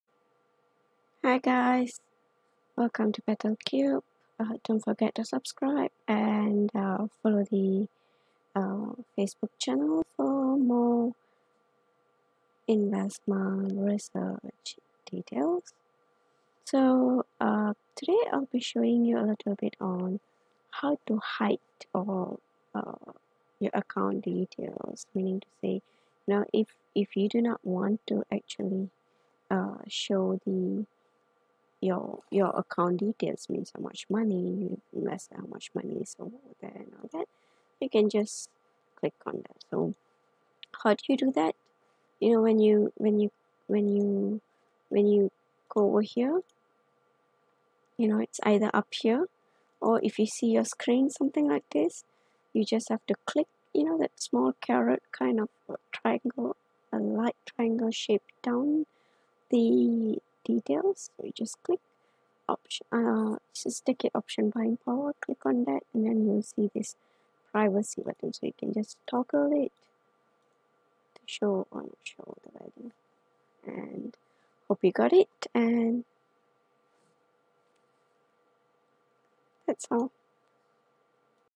TDAmeritrade - HOW TO hide the finance details (privacy setting)
When you want to share your investment, you want to hide your financial detail. How do you do that? This video suggest you how to hide.